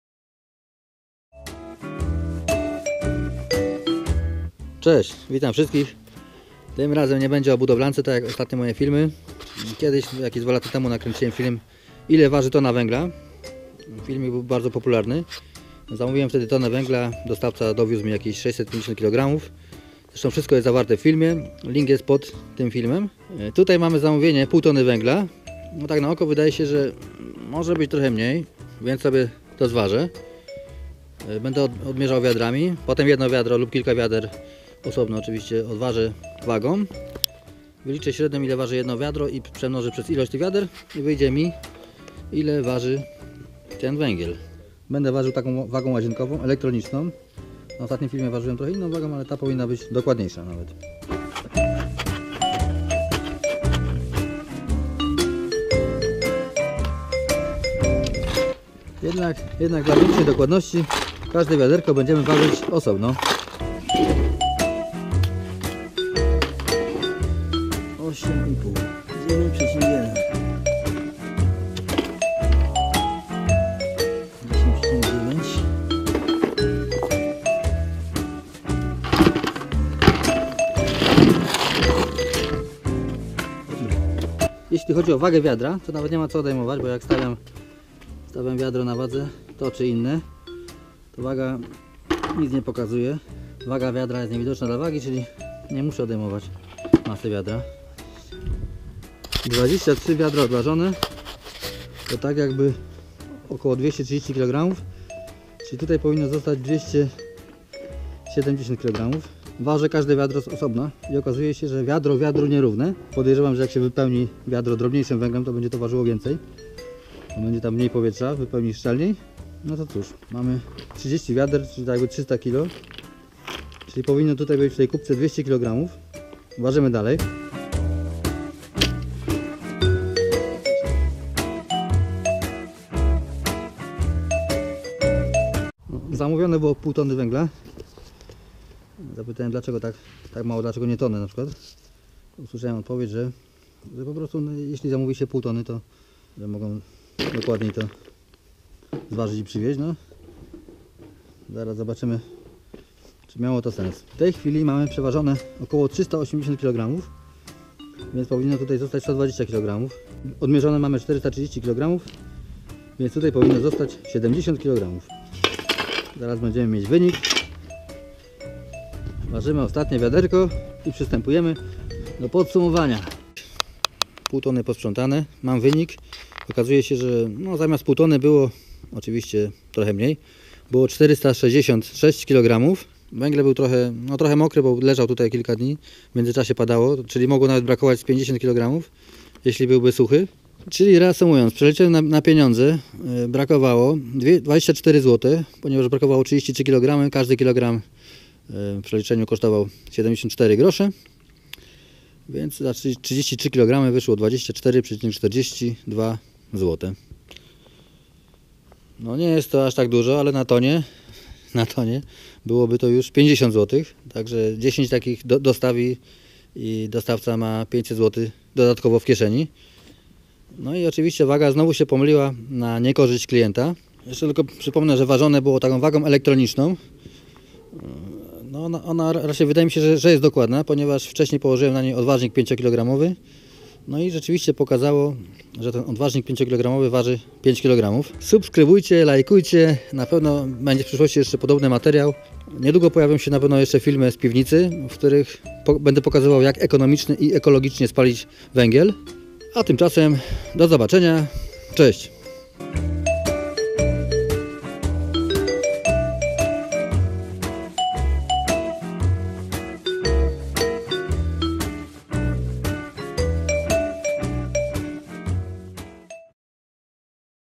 Ile waży pół tony węgla? Zamówiłem pół tony - ile dostałem?
Ile waży Tona węgla ?